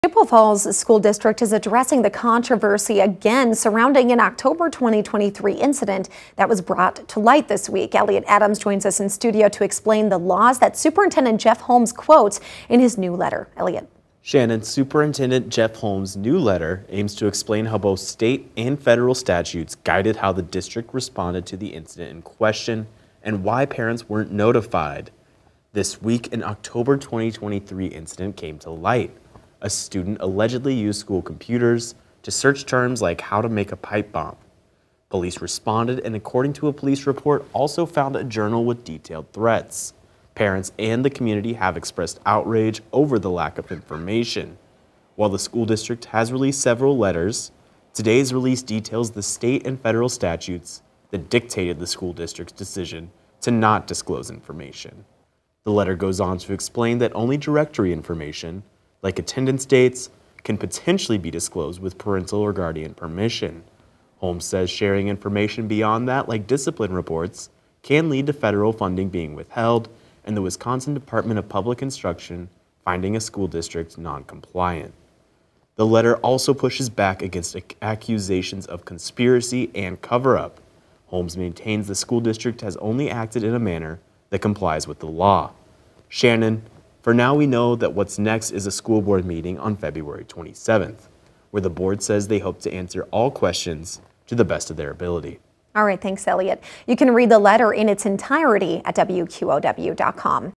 Chippewa Falls Superintendent pens open letter on legality of alerting public to threats
The Chippewa Falls superintendent is once again addressing the controversy surrounding a 2023 incident that was recently brought to light.